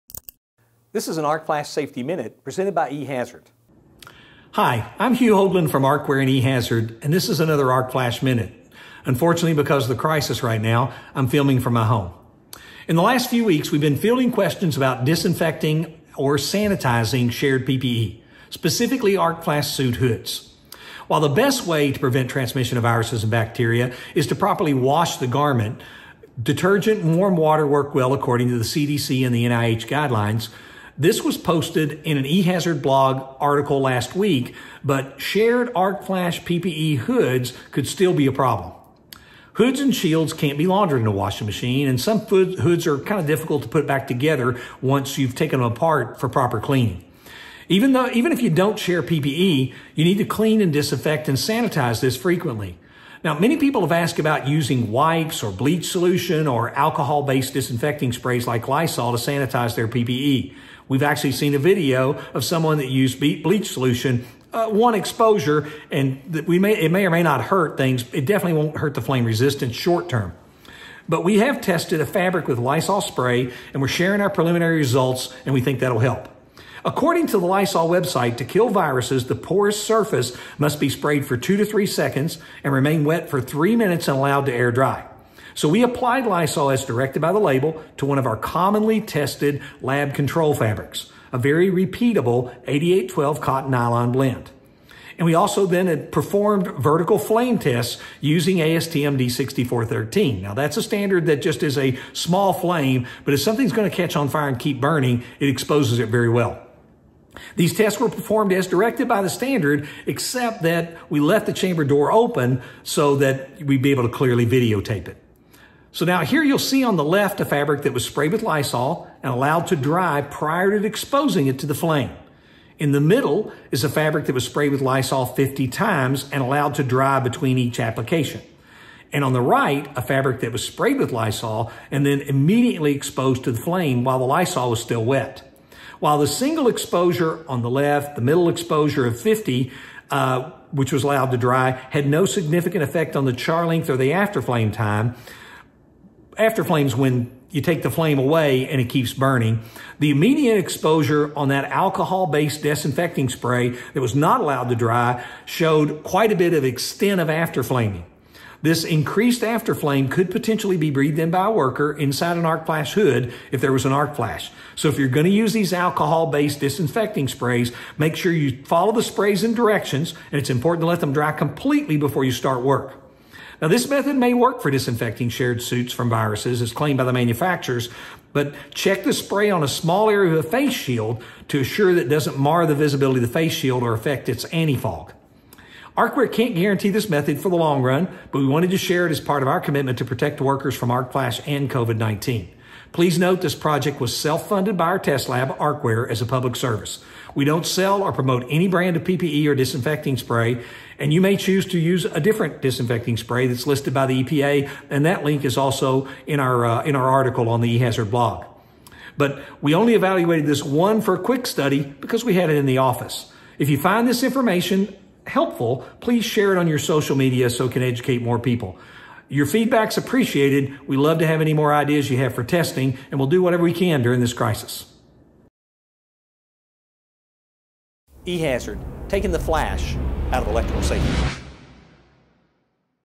Lysol® Test on PPE
Hi, I’m Hugh Hoagland from ArcWear and e-Hazard with an Arc Flash Minute. In the last few weeks, we have been fielding questions about disinfecting or sanitizing shared PPE—specifically, arc flash suits and hoods.  While the best way to prevent transmission of viruses and bacteria is to properly wash the garment (detergent and warm water work well according to the CDC and NIH guidelines